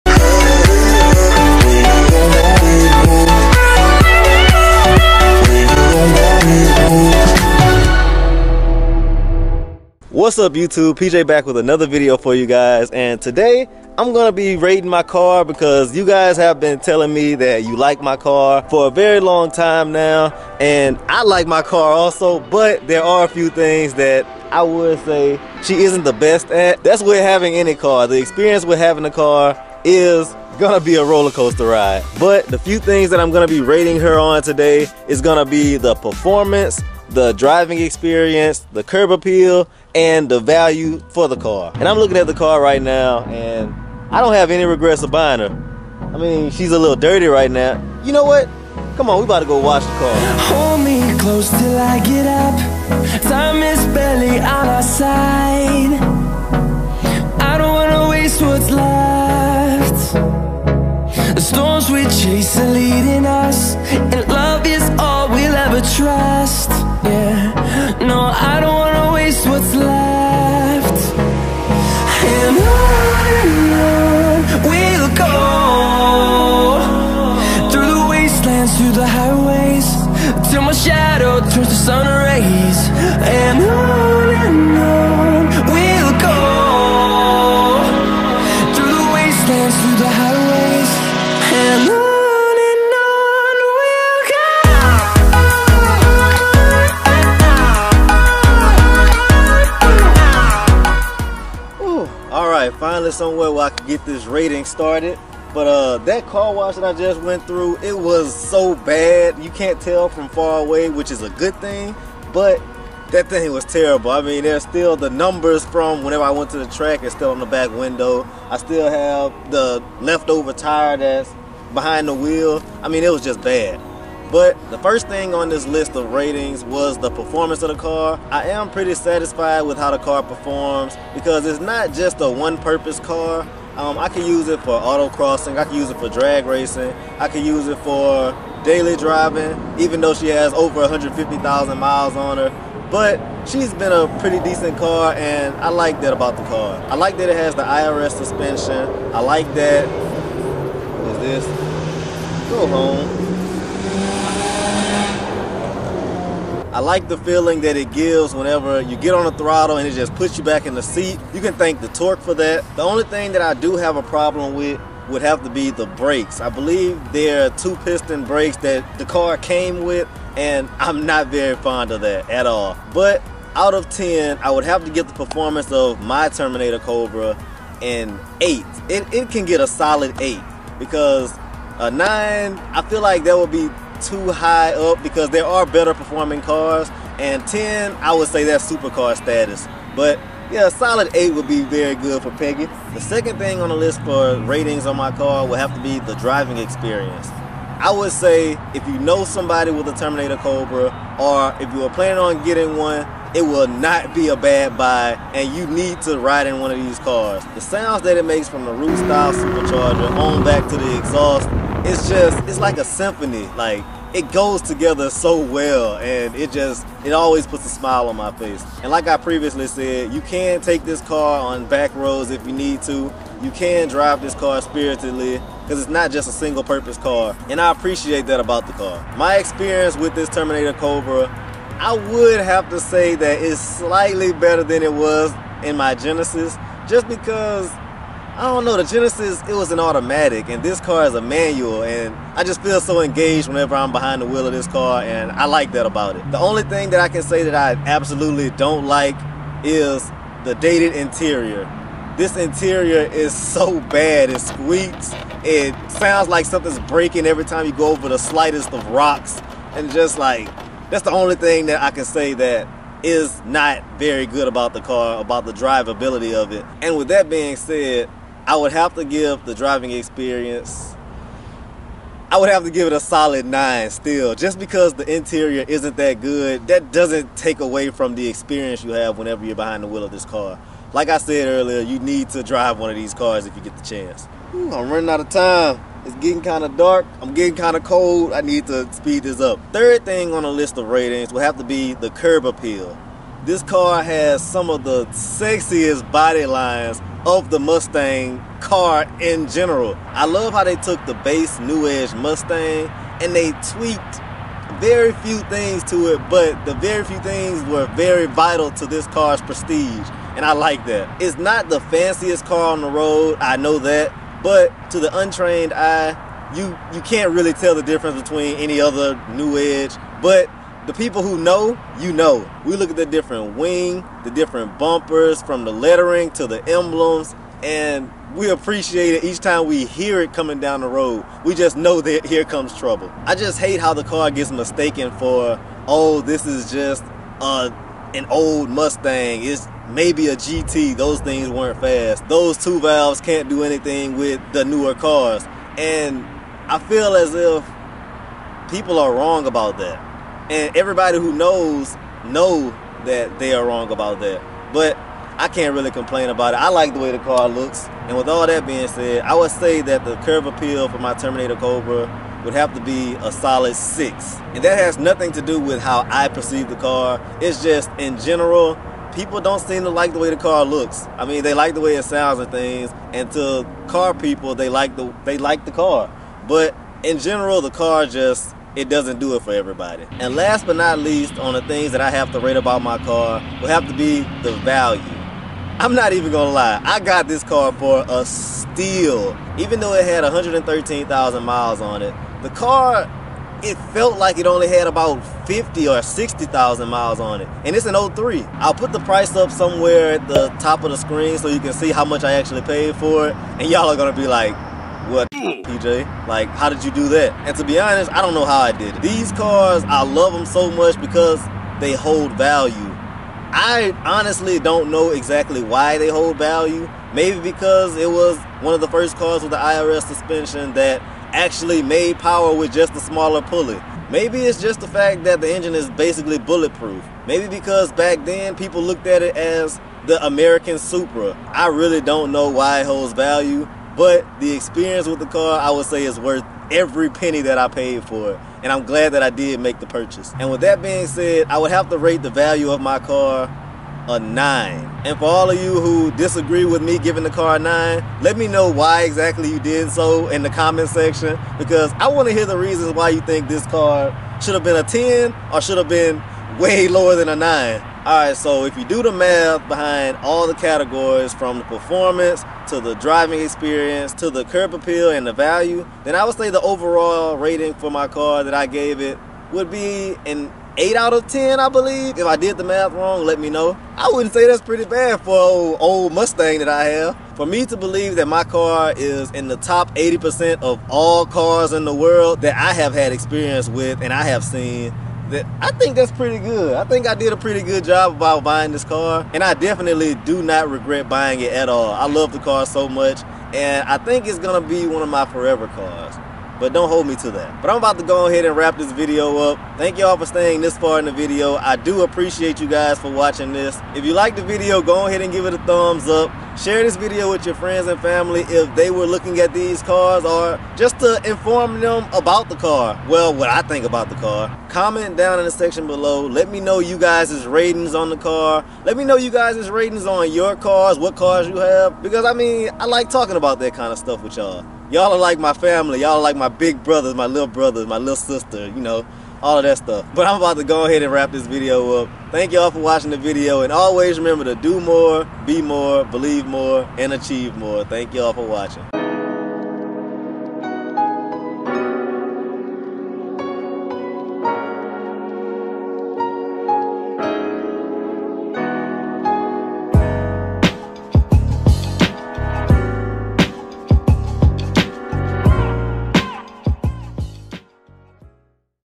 What's the Rating? | Is it Still PHENOMENAL After 20 Years?
I found some free time to rate my car, so I decided to see if my Terminator Cobra was worth the money I paid for it. I divided the ratings into four categories (Performance, Driving Experience, Curb Appeal, and Value). I hope that you agree with how I rate my car!